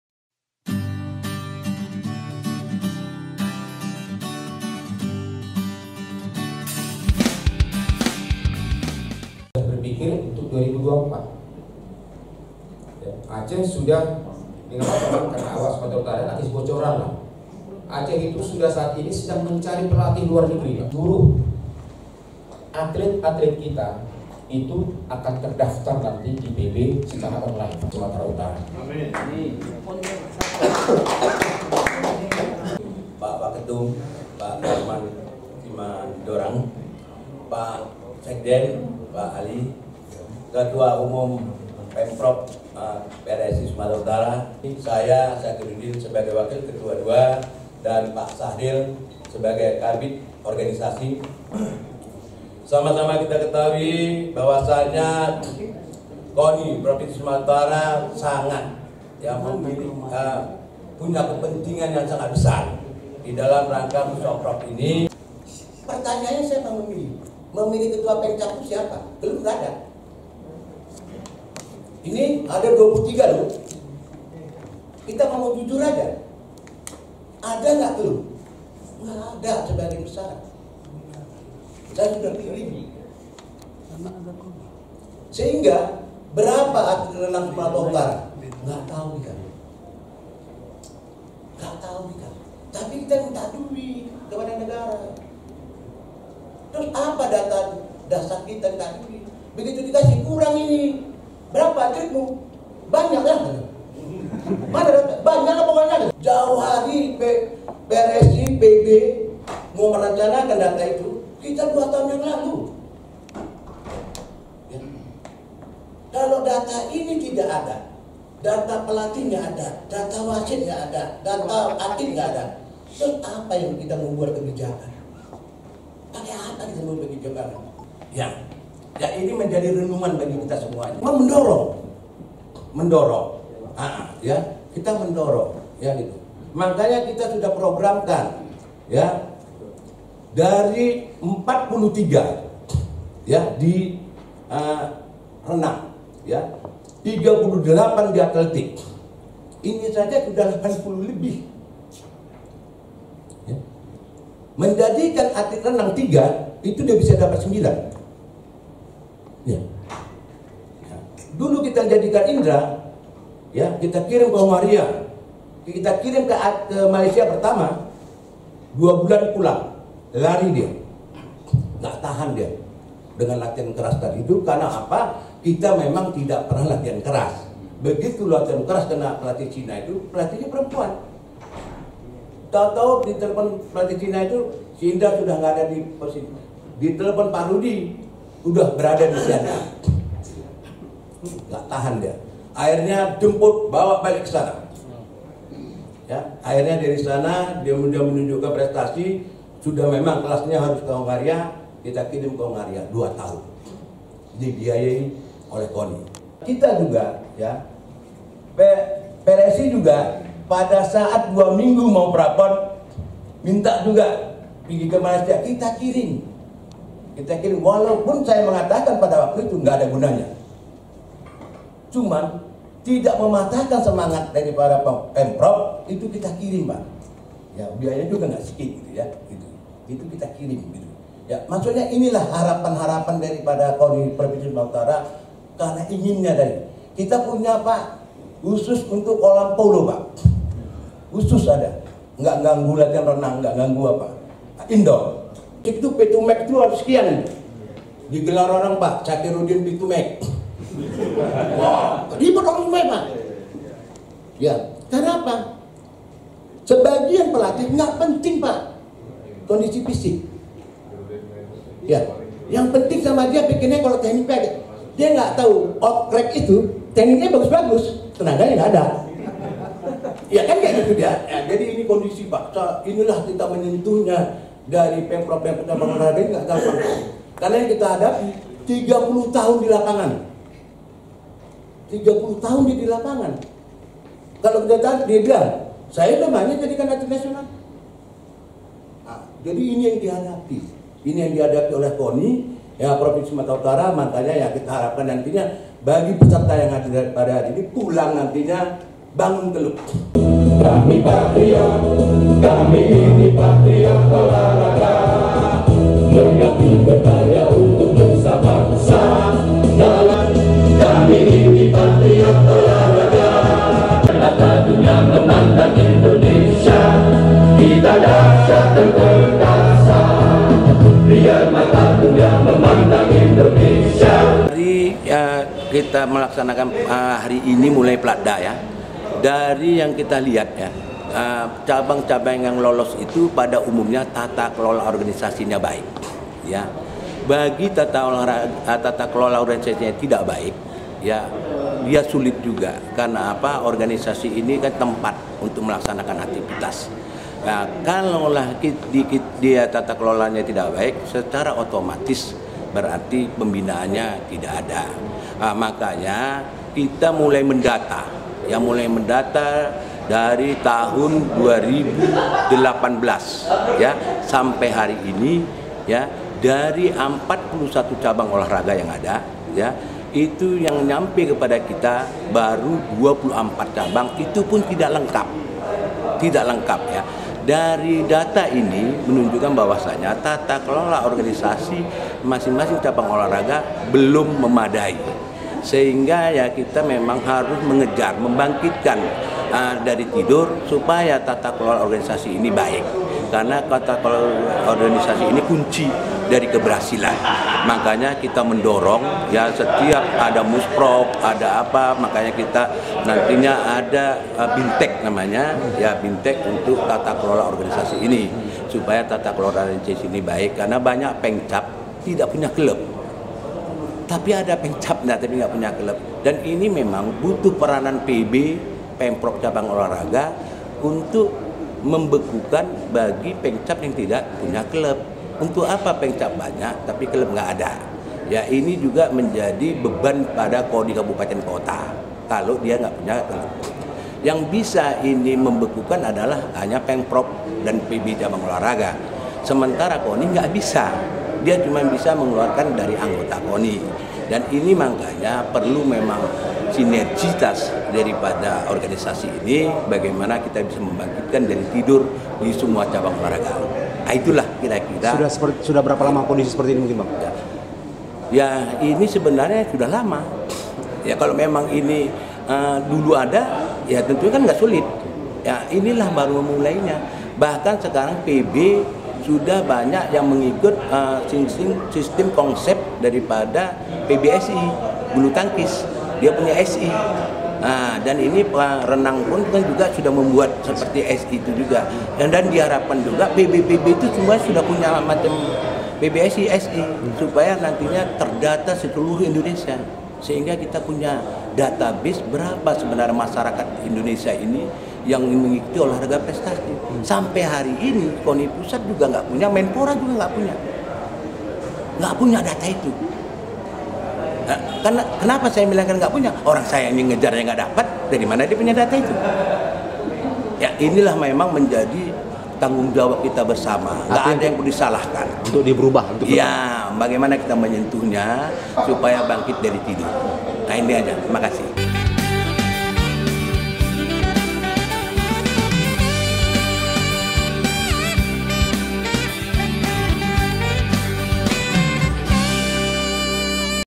Fakta atlit Indonesia versi Koni
Koni Sumatera Utara mengklaim selama ini tata kelola  cabang olah raga di Indonesia masih tertidur. Hal ini menyebabkan tingkat prestasi atlit yang diciptakan dan siap untuk diikutsertakan dalam event olahraga tingkat nasional maupun internasional menurun.

Selain itu Koni juga mengatakan bahwa Pelatih yang ada hanya mengutamakan tehnik dengan mengesampingkan kekuatan. Hal ini terkuak dalam acara Musrov PRSI Sumatera Utara tahun 2020 yang berlangsung di Medan. (putra24jamnewstv)